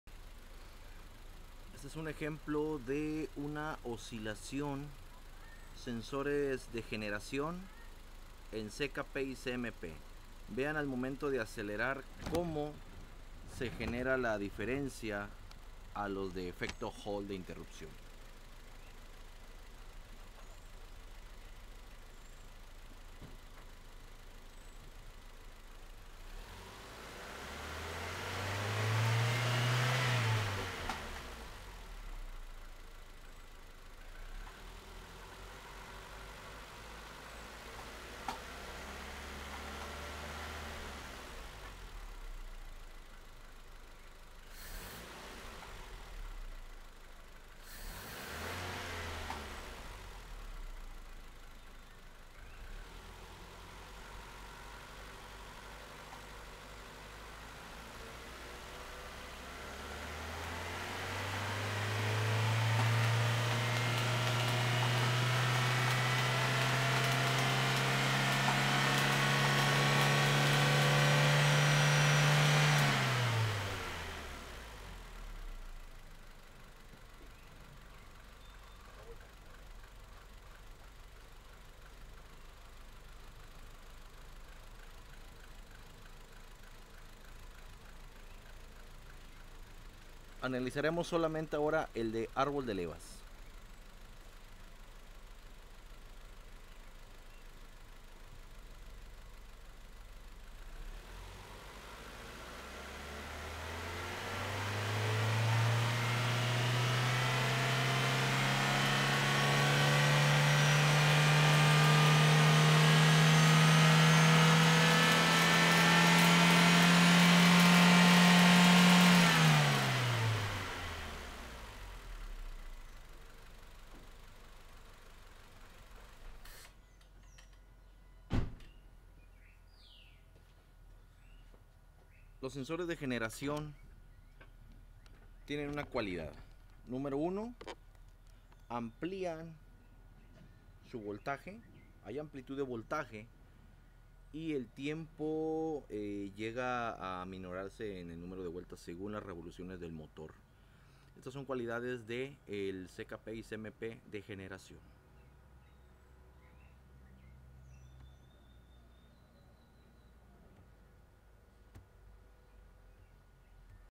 Sincronía motor 1.6 Ford Fiesta 2011
Espero que las señales de los sensores CKP y CMP (arbol de levas y cigueñal) te sirvan como referencia